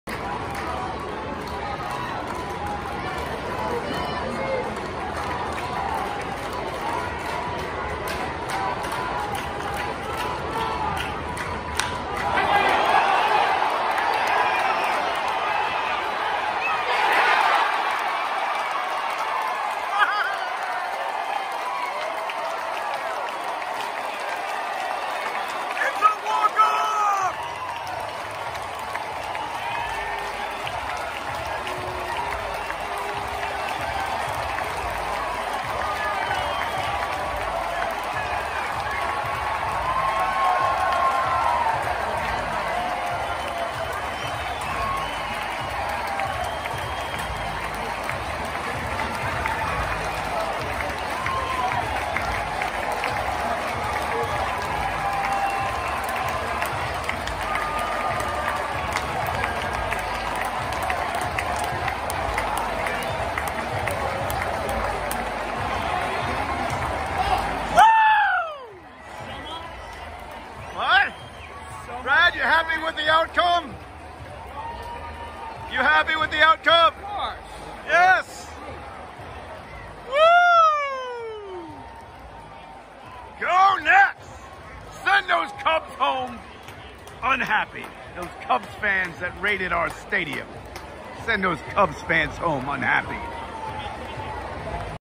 Nationals beat Cubs with a walk-off home run
The Washington Nationals defeat the Chicago Cubs on a Yadiel Hernandez walk-off at home run.
At Nationals Park - Sunday August 1, 2021